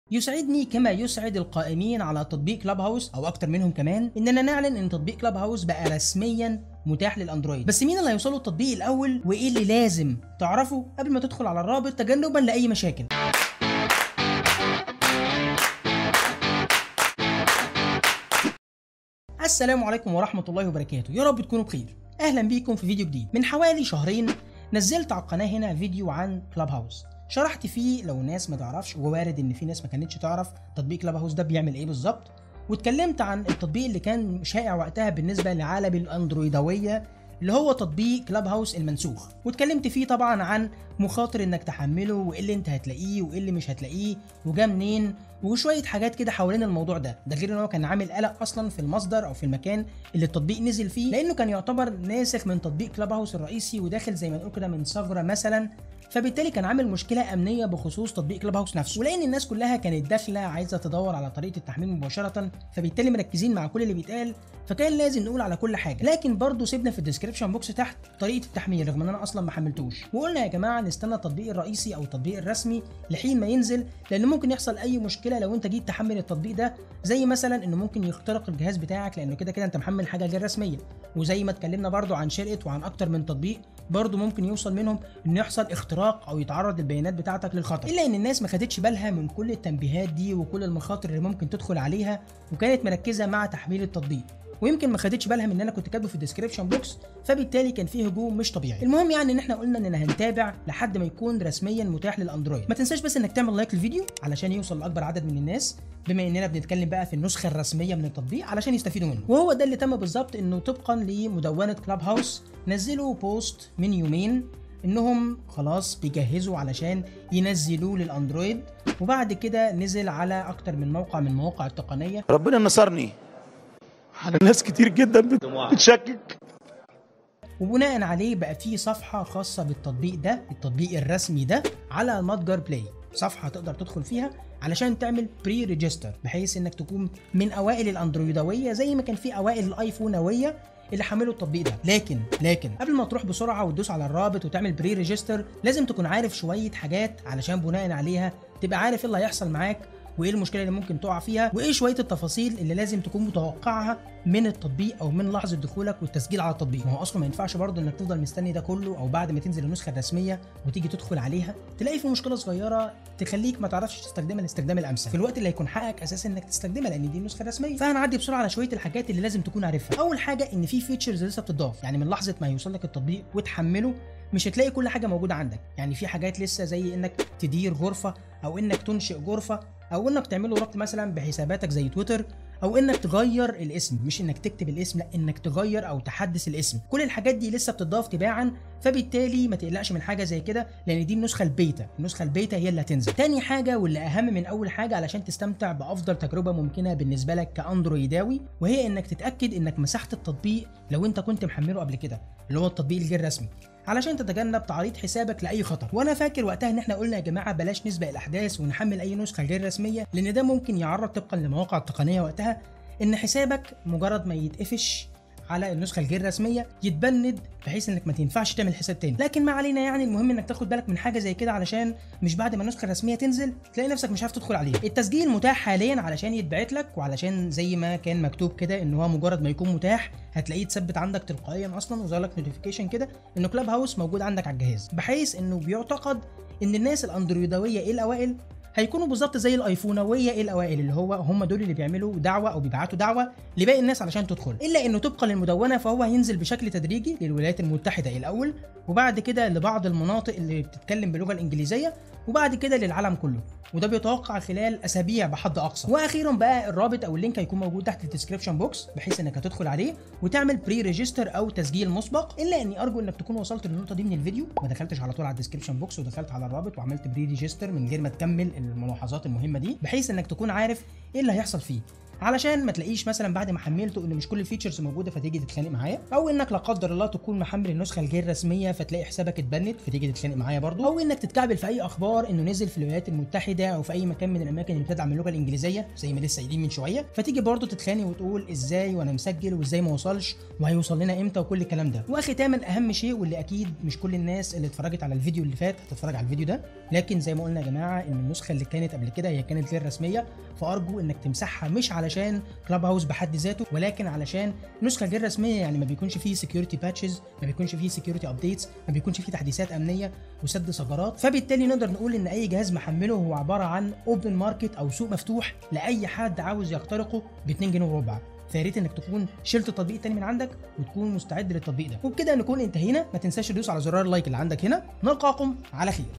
الباحث | رسميًا كلوب هاوس للاندرويد مفتوح للتسجيل| الطريقة وارشادات استخدام!!!
كلوب هاوس رسميًا متاح للاندرويد، تقدر دلوقتي تعرف طريقة التسجيل والارشادات، عشان تتجنب الحظر من الاستخدام، ولازم تفهم شوية حاجات عشان تستخدمه صح.
تطبيق كلوب هاوس تطبيق شات صوتي، ازاي تحمل تطبيق كلوب هاوس للاندرويد، وايه ارشادات كلوب هاوس، عشان تقدر تحمل كلوب هاوس وتستخدمه بافضل شكل، وتكون عارف ايه الطريقة اللي هيشتغل بيها كلوب هاوس ع اندرويد.